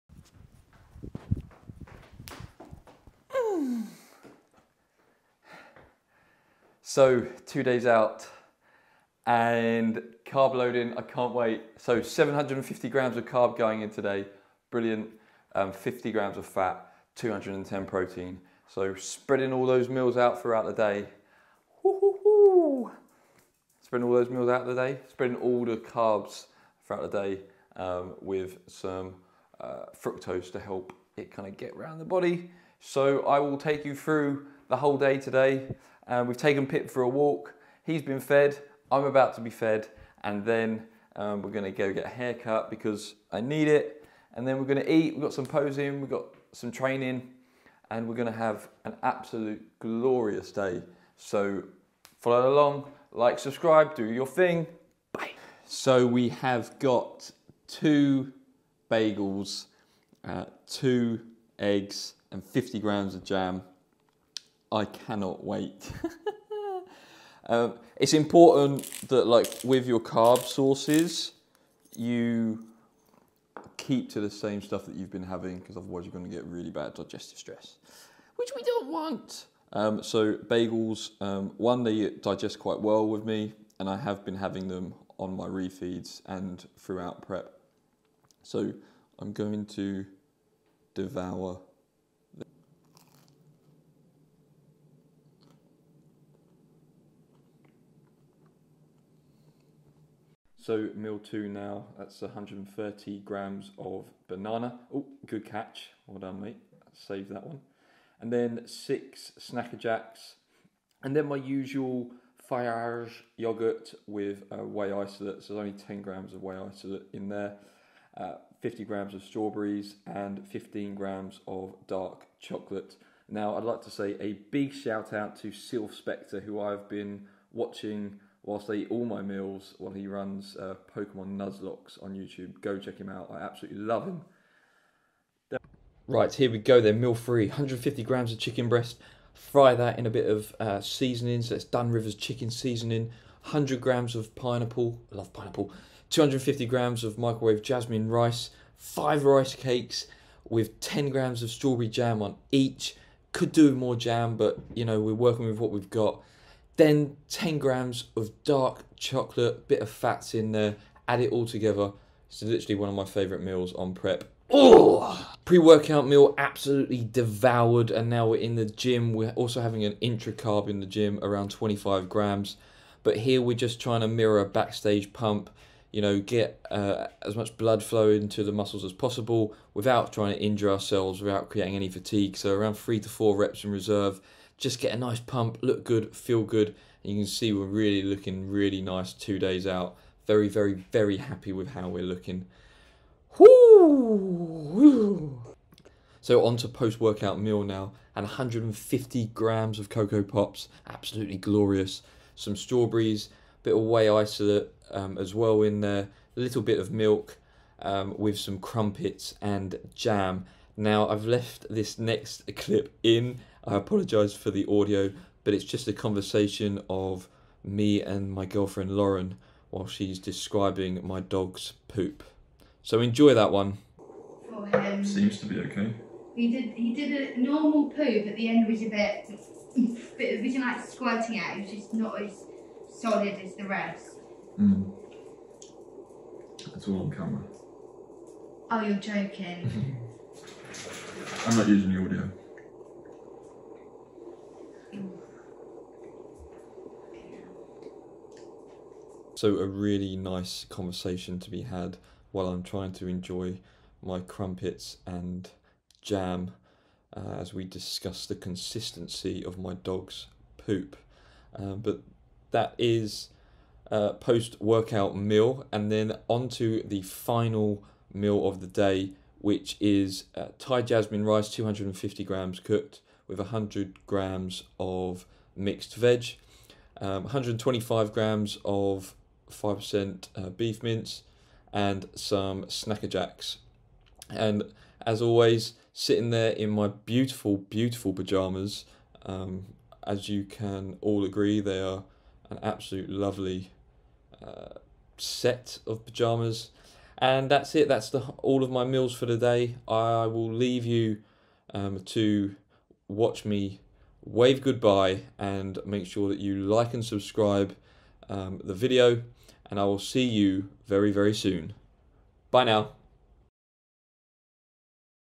Full Day of Eating 750carb | 4,300 calories | Carb Loading 2 days out | UKDFBA BRITISH FINALS
Hi everyone, 2 days out and I begin a relatively aggressive carb load. This is day 1 of my 2 day carb load, 750g of carbs going in. Watch along and enjoy my full day of eating.

Thanks :)